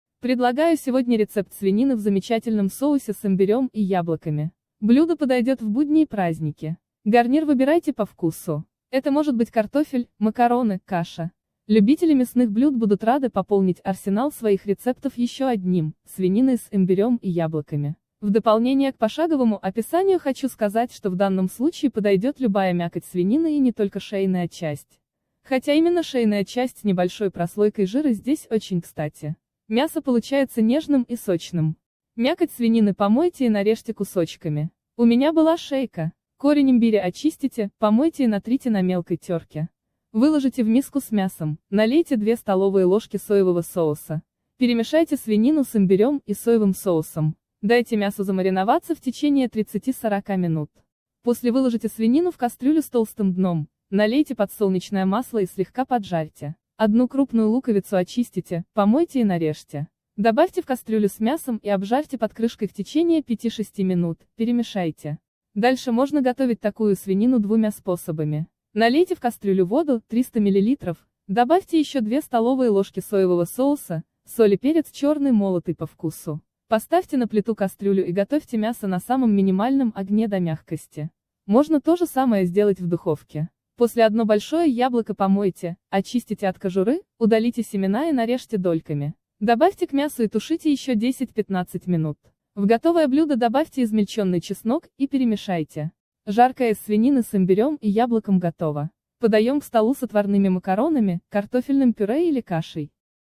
Имбирная свинина с яблоками и луком. Как приготовить рецепт с фото пошагово
Предлагаю сегодня рецепт свинины в замечательном соусе с имбирем и яблоками. Блюдо подойдет в будни и праздники. Гарнир выбирайте по вкусу. Это может быть картофель, макароны, каша.    Любители мясных блюд будут рады пополнить арсенал своих рецептов еще одним: свининой с имбирем и яблоками. В дополнение к пошаговому описанию хочу сказать, что в данном случае подойдет любая мякоть свинины и не только шейная часть. Хотя именно шейная часть с небольшой прослойкой жира здесь очень кстати. Мясо получается нежным и сочным.

Ингредиенты:
Свиная шейка — 500 Грамм,
Корень имбиря — 10 Грамм,
Соевый соус — 3-4 Ст. ложек,
Яблоко — 1 Штука,
Лук репчатый— 1 Штука,
Подсолнечное масло — 30 Миллилитров,
Соль — 0,5 Чайных ложки,
Перец черный молотый — 1/4 Чайных ложки,